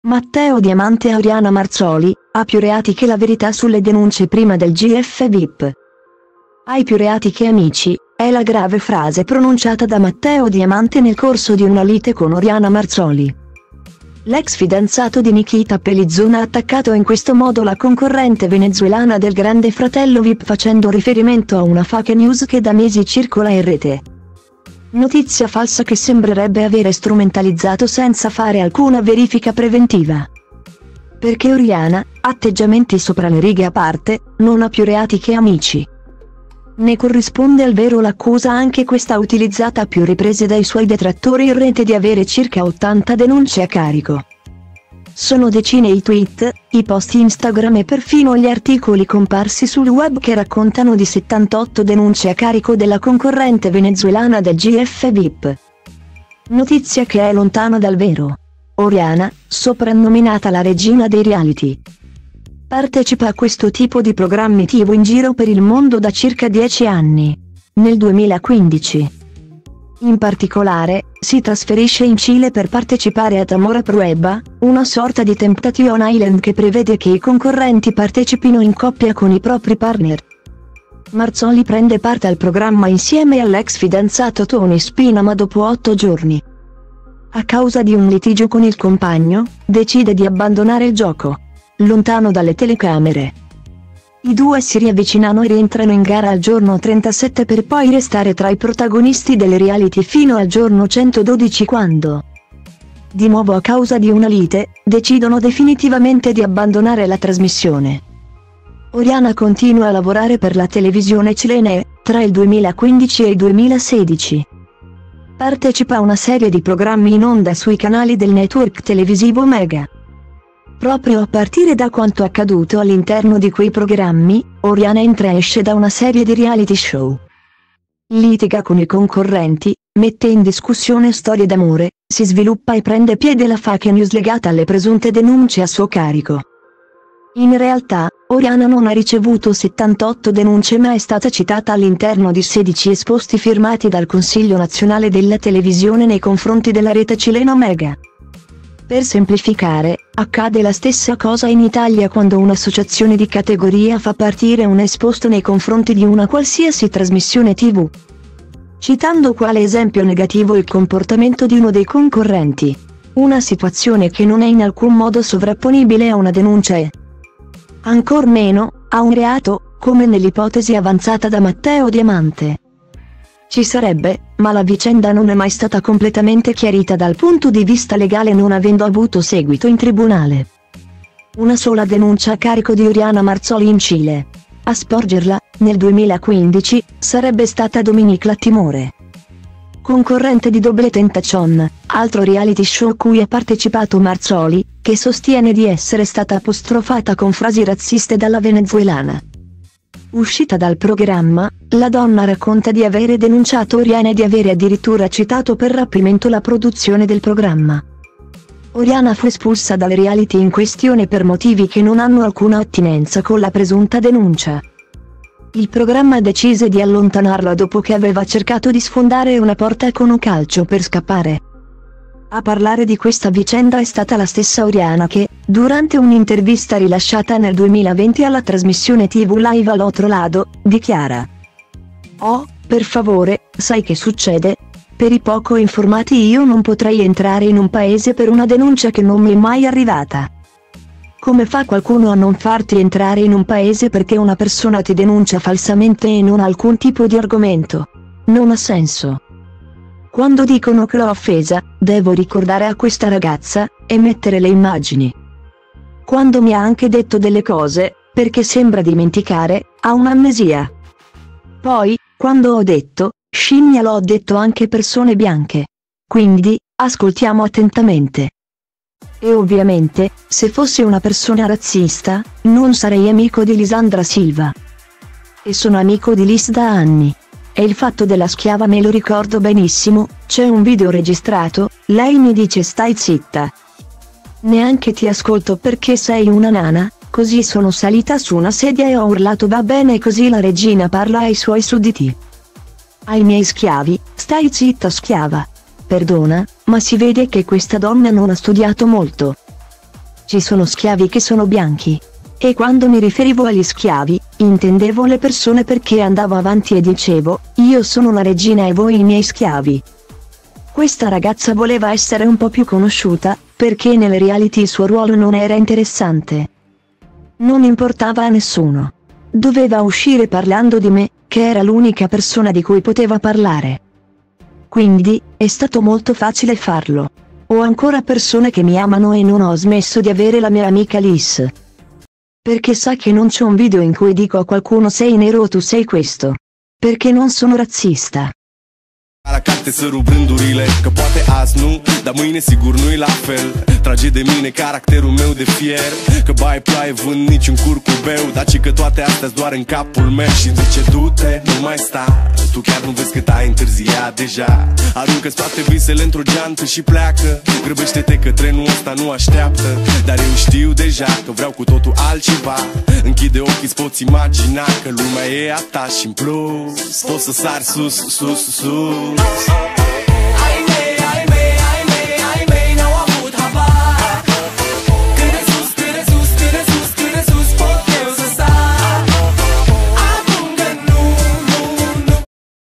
Matteo Diamante a Oriana Marzoli: “Ha più reati che la verità sulle denunce prima del GF Vip
“Hai più reati che amici”: è la grave frase pronunciata da Matteo Diamante nel corso di una lite con Oriana Marzoli.L’ex fidanzato di Nikita Pelizon ha attaccato in questo modo la concorrente venezuelana del Grande Fratello Vip facendo riferimento a una fake news che da mesi circola in rete,notizia falsa che sembrerebbe avere strumentalizzato senza fare alcuna verifica preventiva.